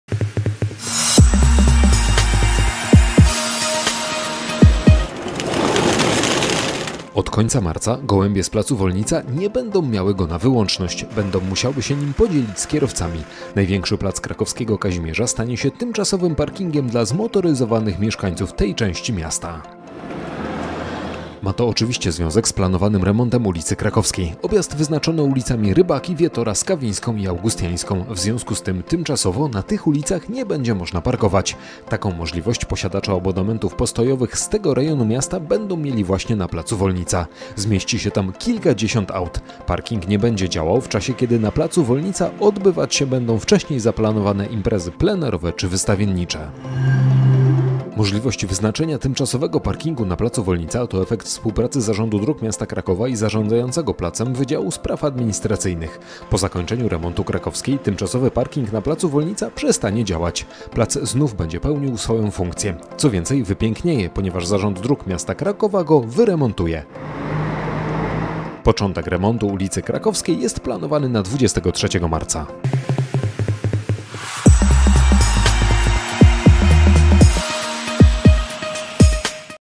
Gdzie zostawić samochód podczas remontu ul. Krakowskiej?
Gdzie zostawić samochód podczas remontu ul. Krakowskiej? Takie pytanie stawiają sobie zmotoryzowani mieszkańcy tej części Krakowa. Odpowiedzią jest plac Wolnica. Jak informuje Zarząd Dróg Miasta Krakowa, na czas remontu zostanie on udostępniony mieszkańcom.